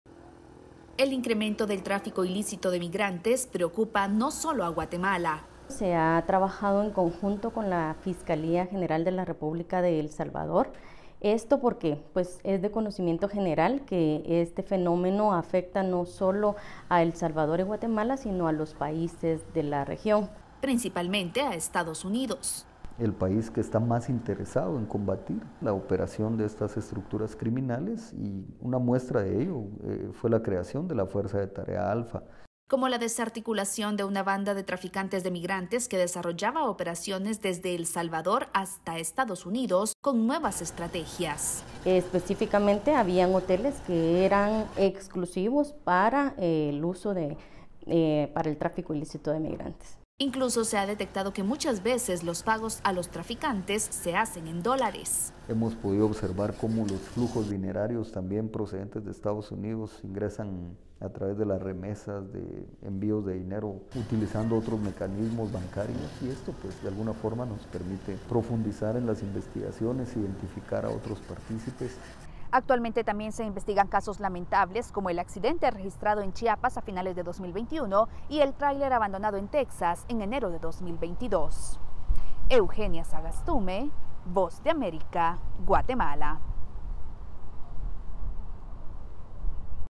Investigaciones conjuntas contra el tráfico de inmigrantes
Estados Unidos apoya a autoridades centroamericanas para investigar bandas dedicadas a trasladar a migrantes de manera irregular a cambio de dinero. Eugenia Sagastume desde Guatemala explica cómo las fiscalías trabajan para erradicar el fenómeno.

¡Visítenos!